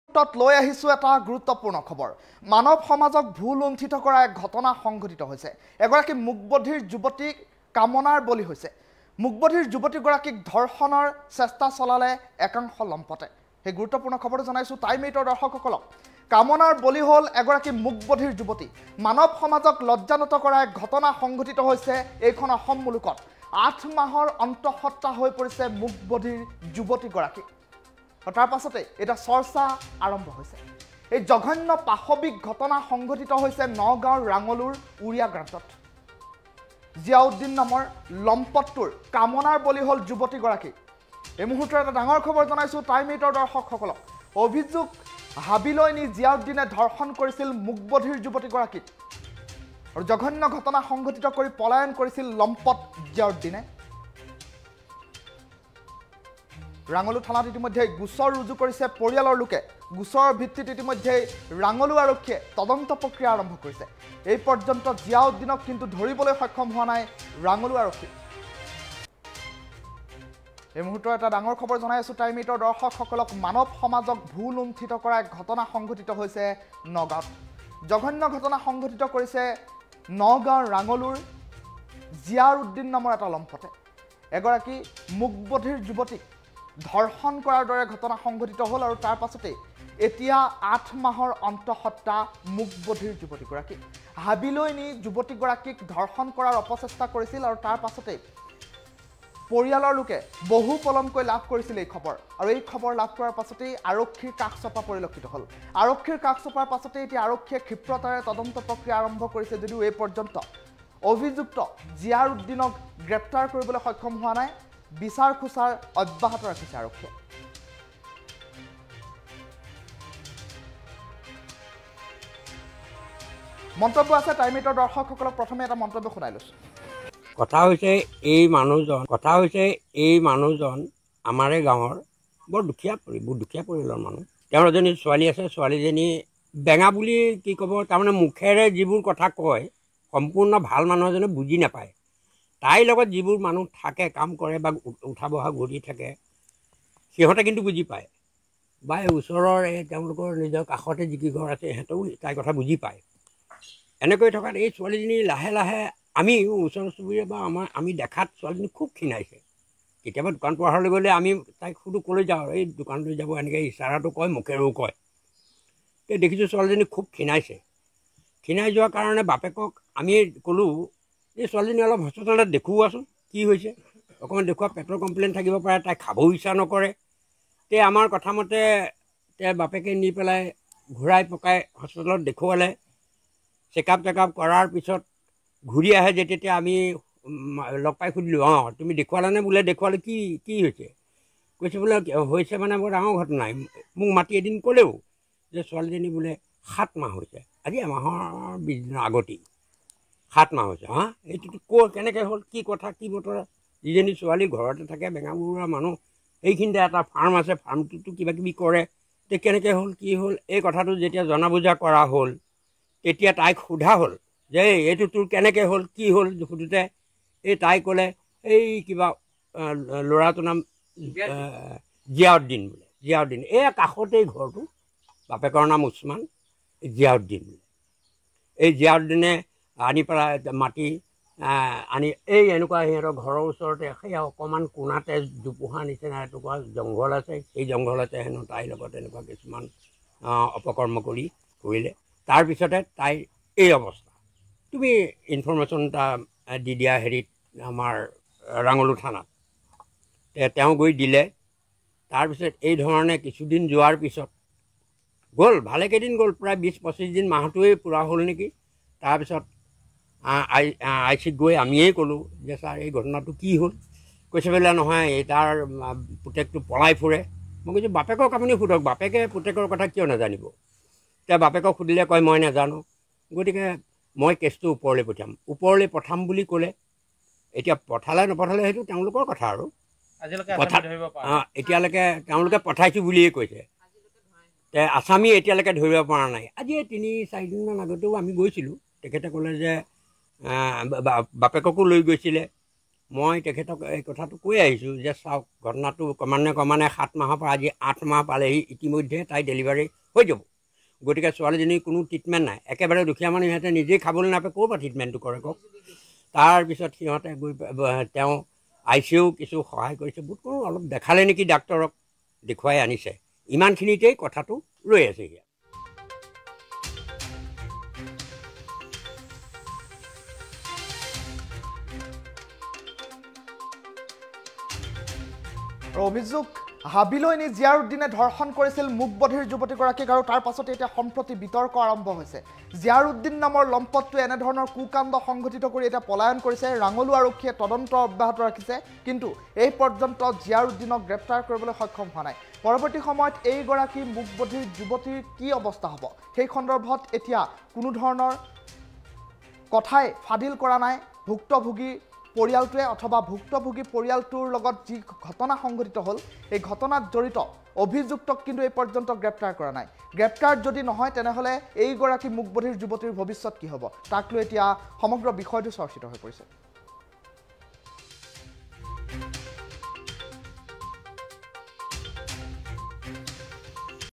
৮ মাহৰ অন্তঃসত্ত্বা হৈ পৰিল মুকবধিৰ যুৱতীগৰাকী
৮ মাহৰ অন্তঃসত্ত্বা হৈ পৰিল মুকবধিৰ যুৱতীগৰাকী, জিয়াউদ্দিন নামৰ লম্পটৰ কামনাৰ বলি যুৱতীগৰাকী